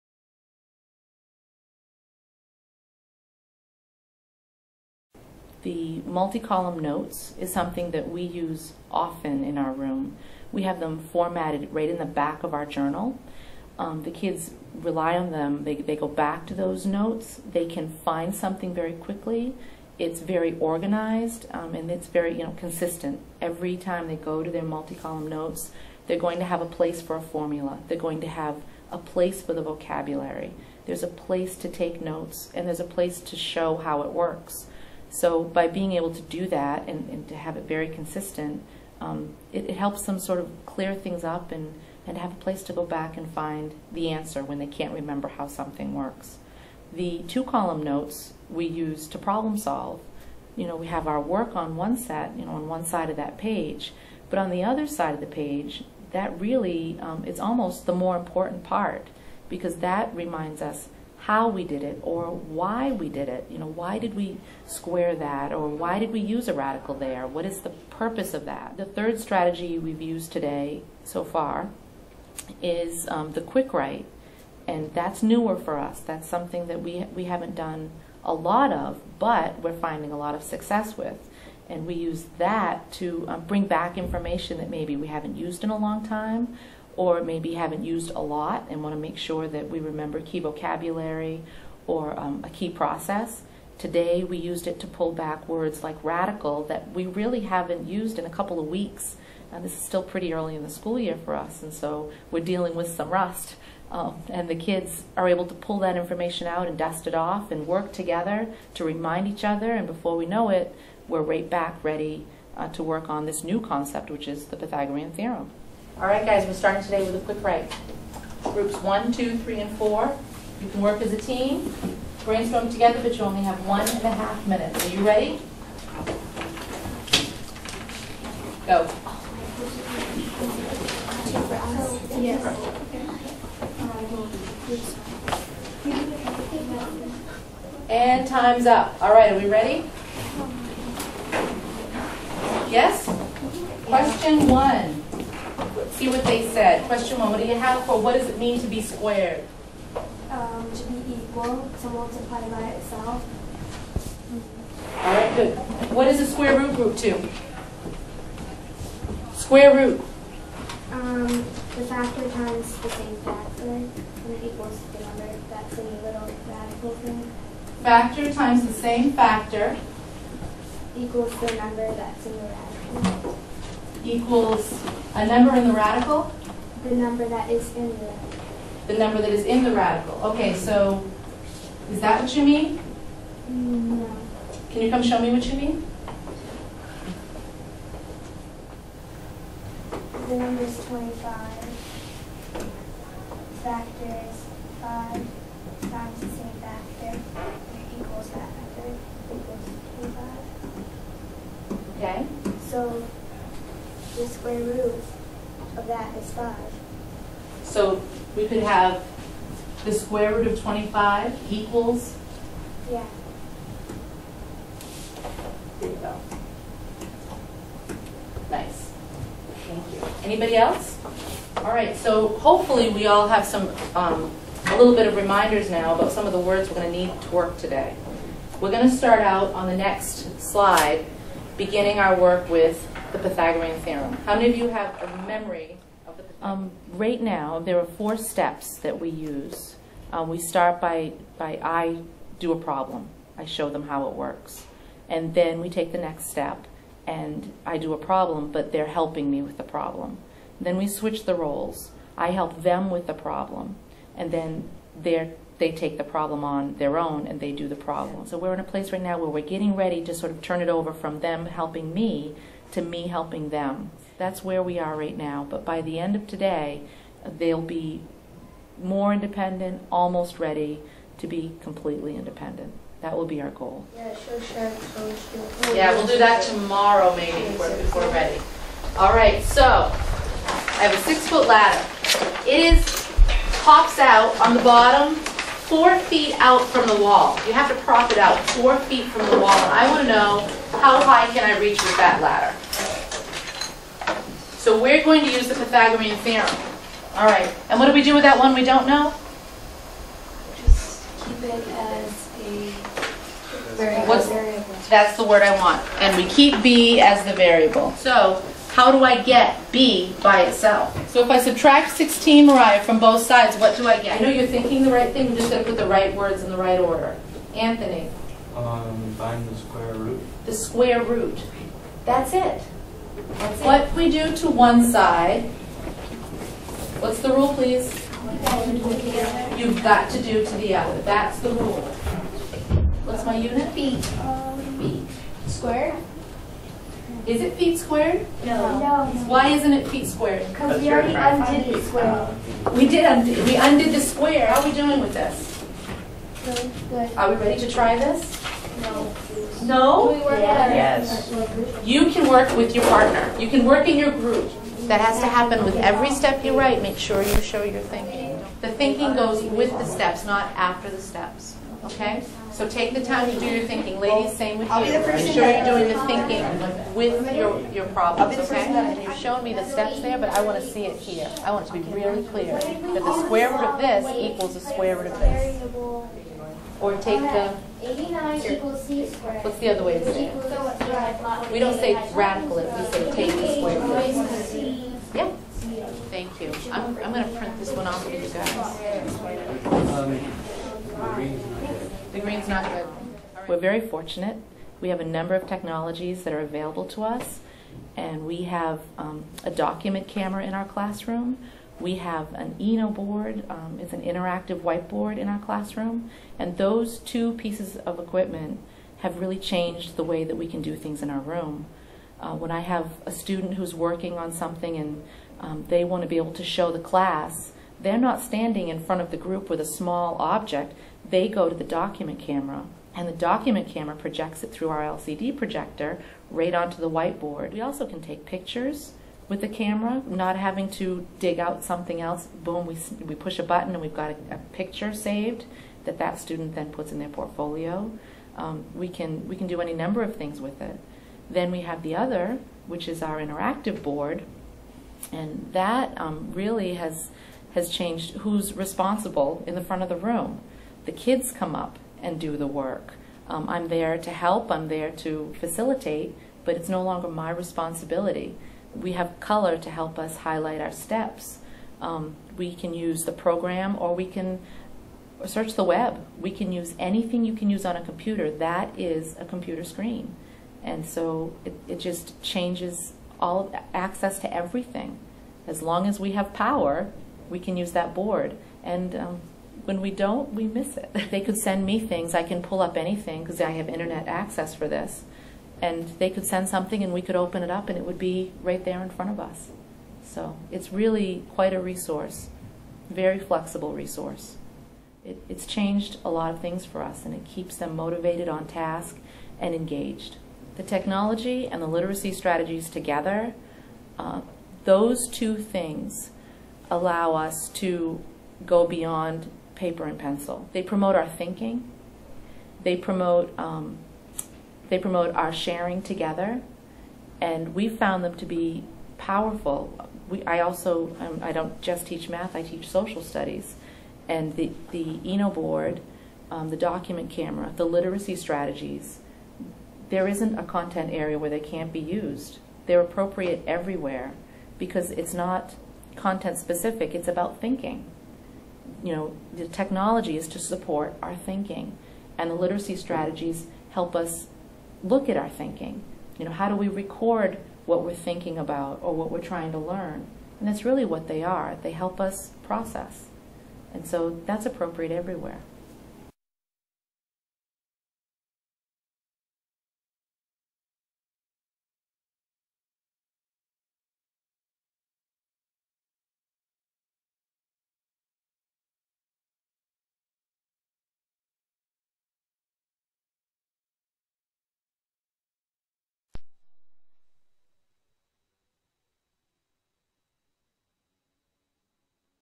Two and Three Dimensional Shapes
Here, a middle school math teacher demonstrates multiple literacy strategies in a single class to support and extend learning. She utilizes Multi-Column Notes to help students organize new information about mathematical concepts. The instructor combines Two-Column Notes with a Triple-Entry Vocabulary Journal in a way that engages students both in the organizing of information and the capacity to reflect on new learning. She also uses Quick Writes to help students build background knowledge and to consolidate learning.